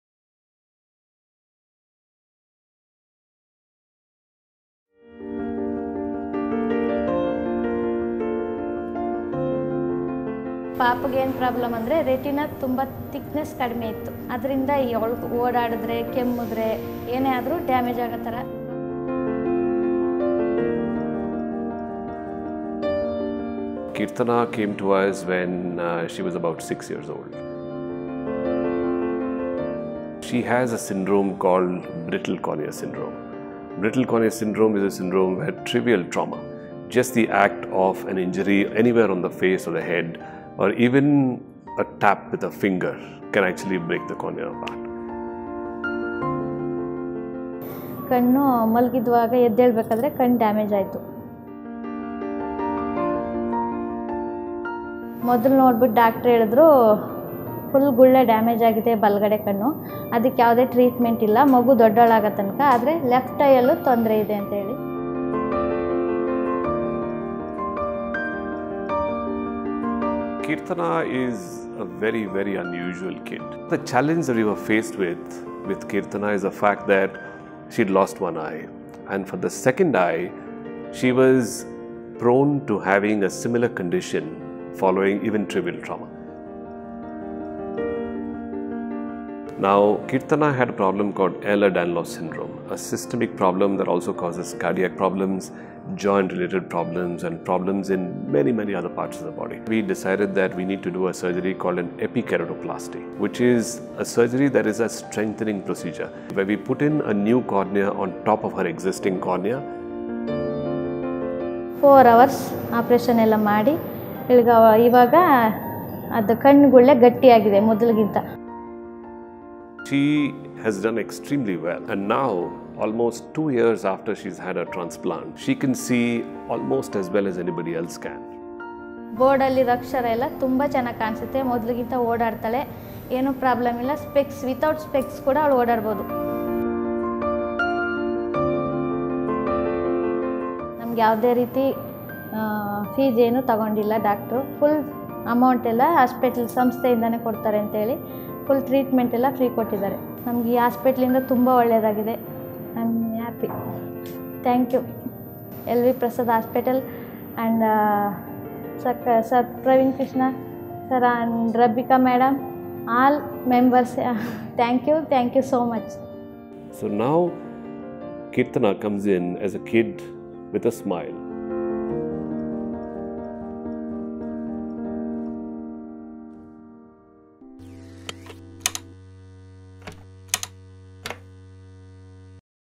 Life Changing Brittle Cornea Syndrome Treatment | Tej Kohli Foundation
Keerthana and her mother received free treatment at the Tej Kohli Cornea Institute.  They first came to the Tej Kohli Cornea Institute at the LV Prasad Hospital when Keerthana was just six years old.  She has a syndrome called Brittle Cornea Syndrome which means that every small action could damage her eyes.  Her right eye was too damaged for treatment, but a strengthening procedure was carried out on her left eye, where a new cornea was put in on top of her existing cornea.  Two years later, she can see almost as well as anyone else can even without wearing glasses.